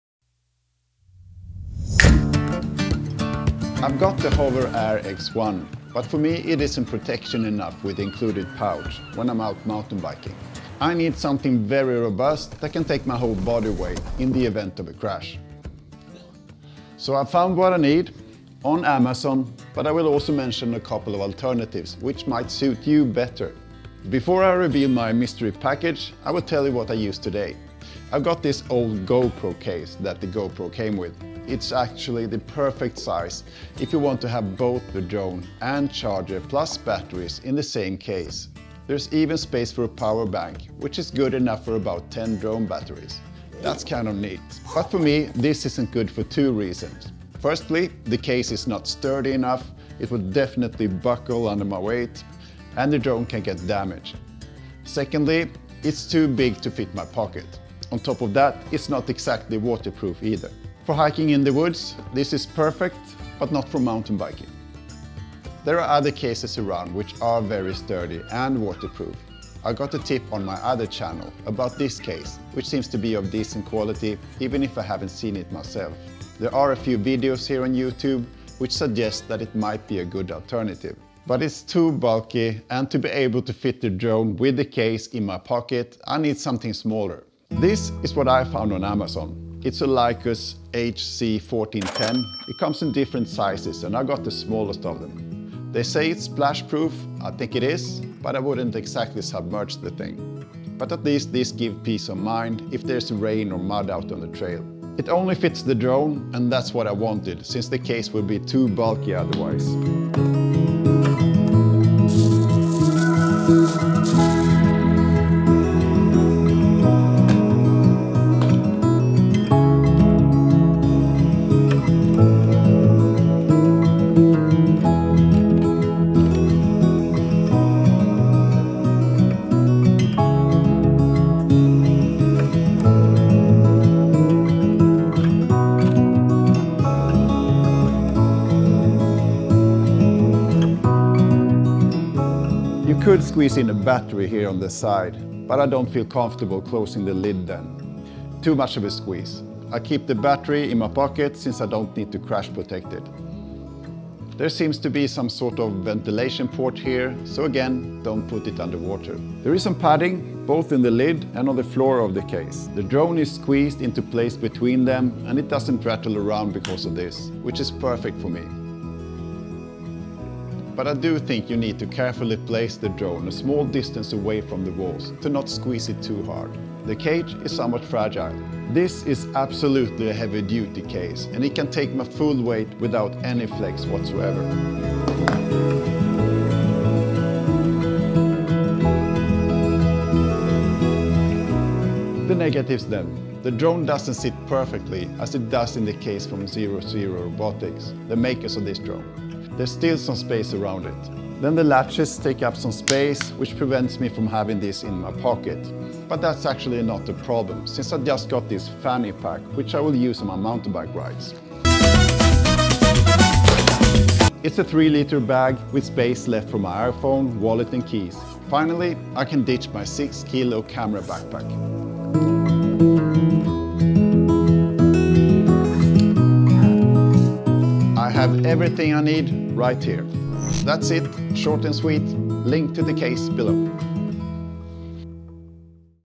TINY ROBUST hard case for the Hover AIR X1 | CRASH PROOF | Lykus HC-1410 REVIEW
This is the best hard case I could find for the Hover Air X1 drone. It can take a crash and it's also very cheap. Lykus HC-1410 REVIEW.

► X-tra Batteries

0:00 Intro
0:25 Gopro case
1:05 Pelican 1040
1:27 Lykus HC-1410
3:00 Standing on it
3:12 The negatives